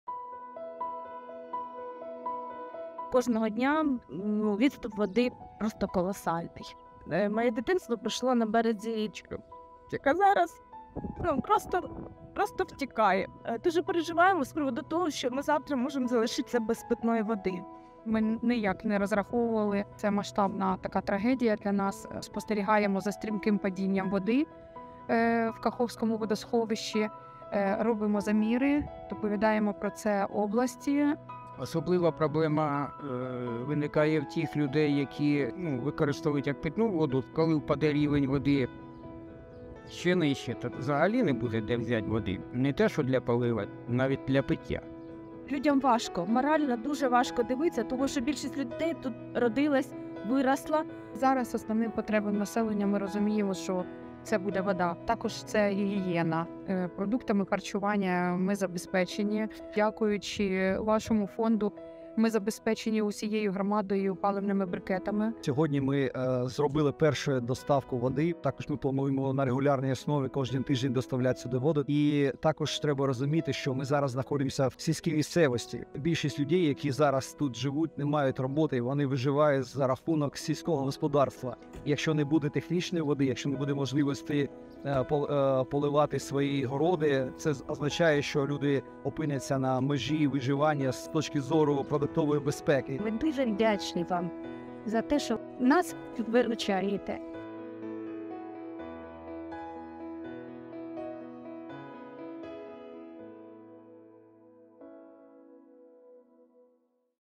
Jak pomáháme u Kachovské přehrady?
Po protržení přehrady Nová Kachovka dodáváme místním lidem pitnou vodu, hygienické potřeby a potraviny. Většina lidí v oblasti je závislá na zemědělství, budeme je proto podporovat, aby nepřišli o zdroj obživy.
Děkujeme, že jste s námi!